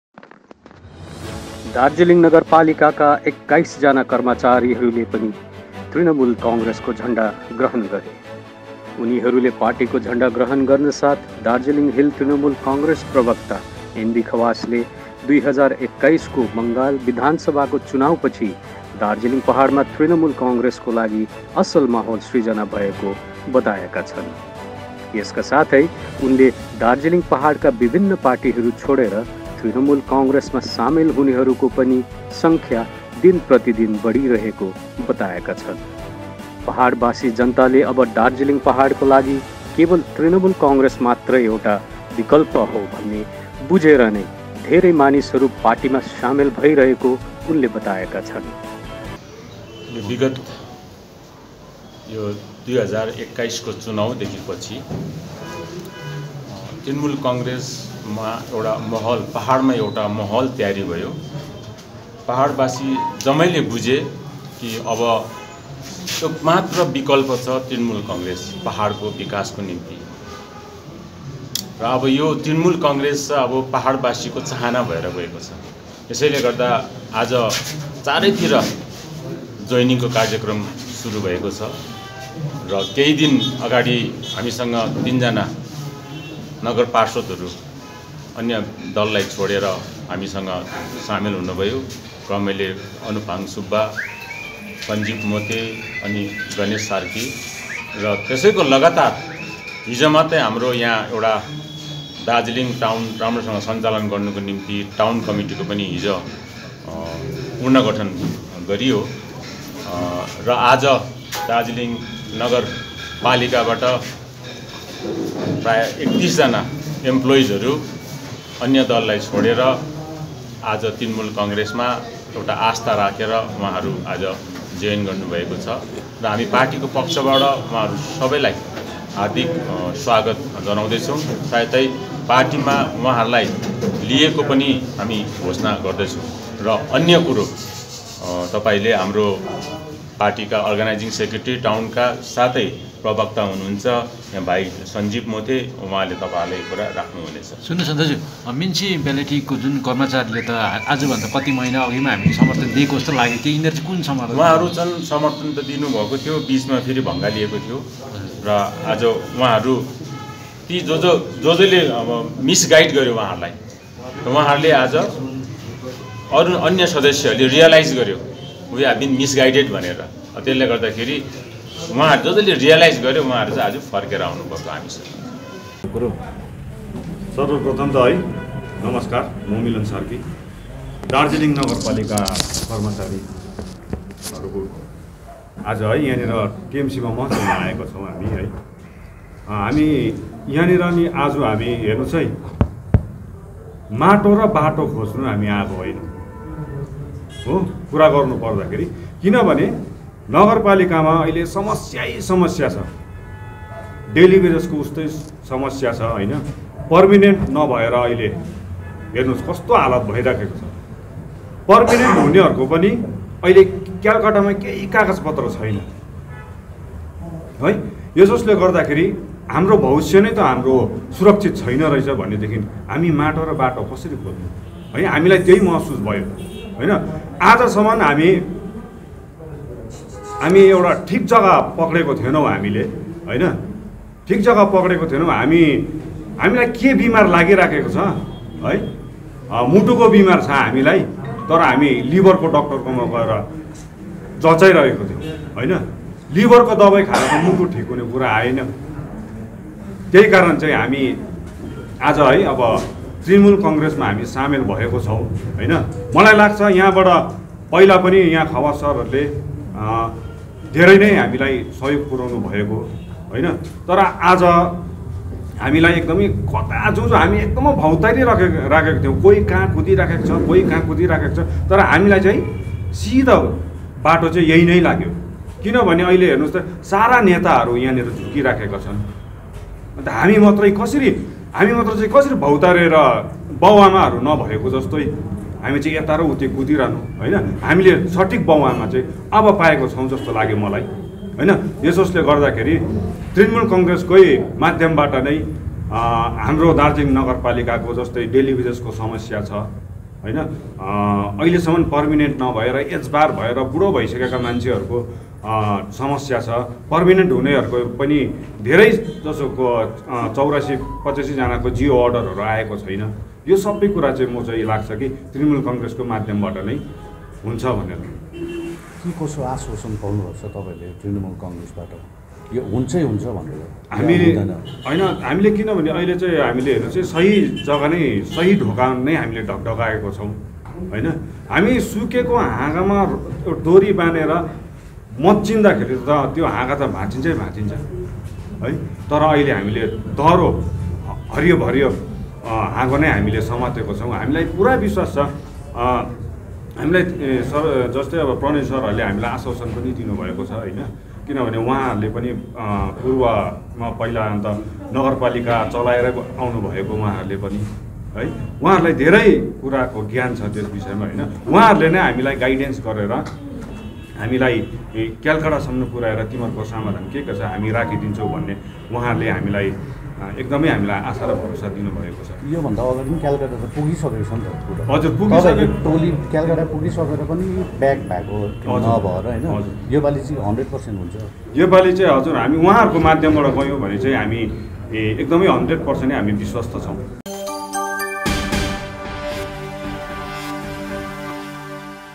21 DARJ MUNICIPALITY STAFF JOIN TMC
TMC spokesperson NB Khawas claims party faring well in Hills after Assembly elections